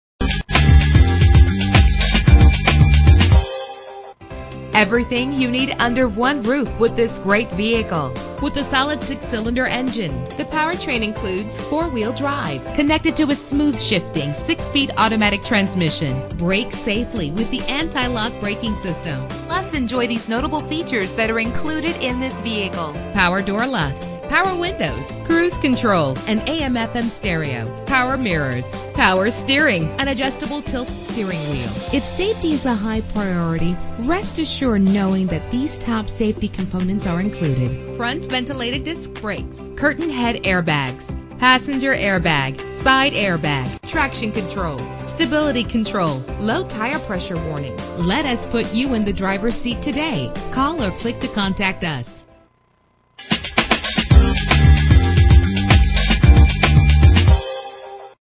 2014 RAM 3500 - Ocala FL
Year: 2014
Make: RAM
Model: 3500
Trim: Tradesman
Engine: 6.7 liter inline 6 cylinder 24 valve
Transmission: 6-Speed Automatic
Color: Bright White
Mileage: 0
Address:
3440 South Pine Avenue